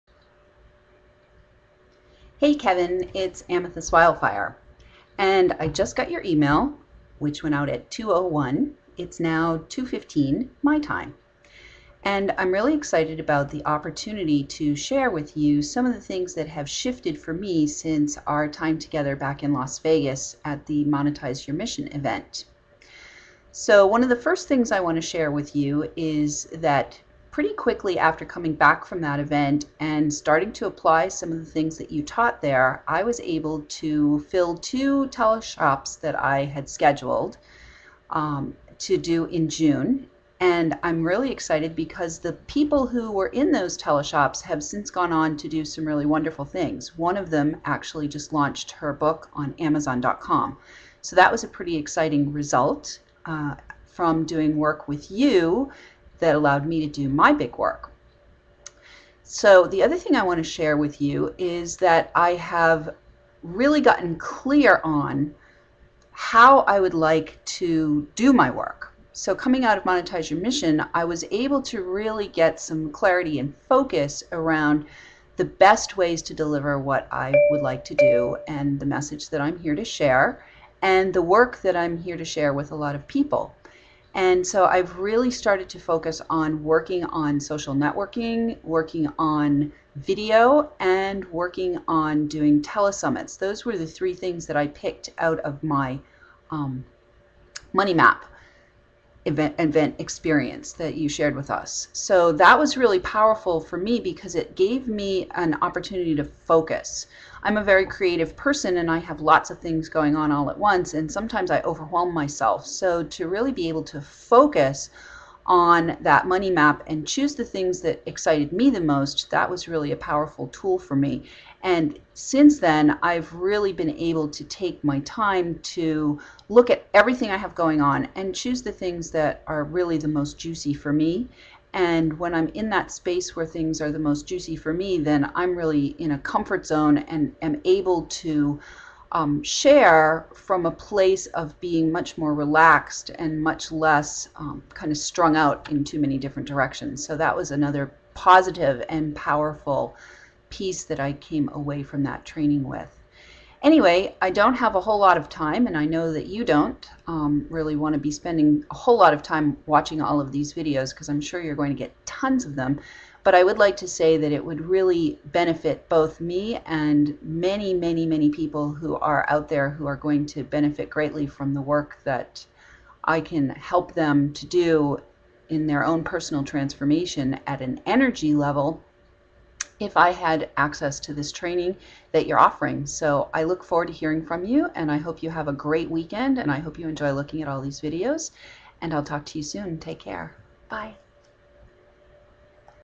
PICK ME!! Kevin Nations with Love from Amethyst Wyldfyre
Video submission to Kevin Nations by Amethyst Wyldfyre for Training in Columbus Ohio I'd LOVE To come to Columbus and DISCOVER the Craft of building a big ticket biz with you!